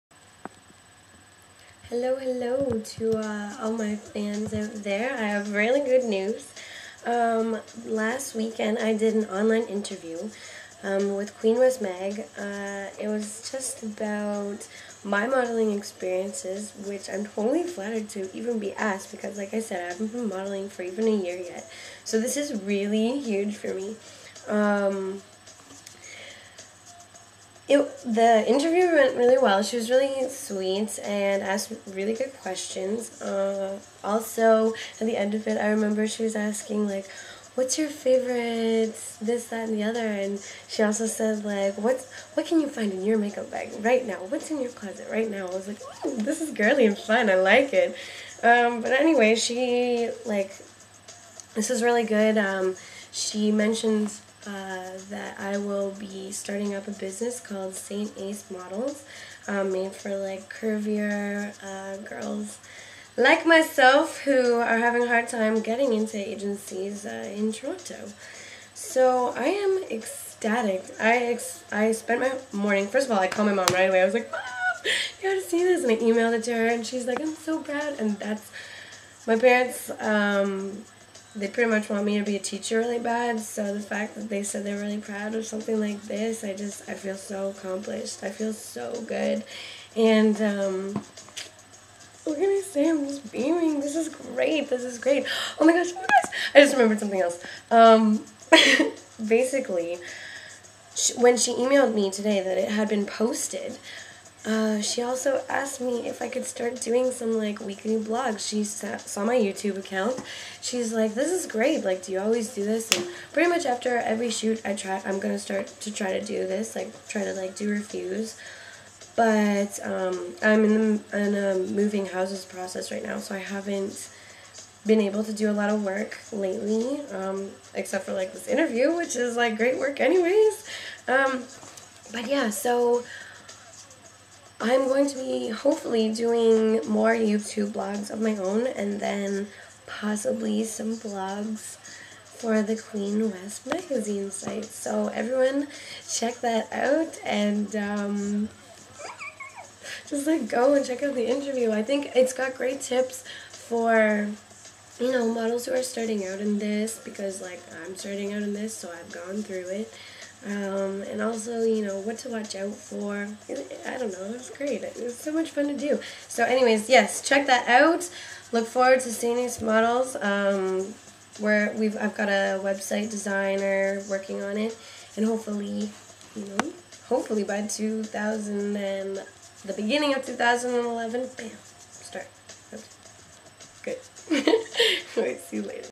Interviewed with Queen West Magazine
Back when I was "The Model Next Door" I was chosen to be interviewed by a new magazine. Here's my thoughts after being interviewed. *I was excited!*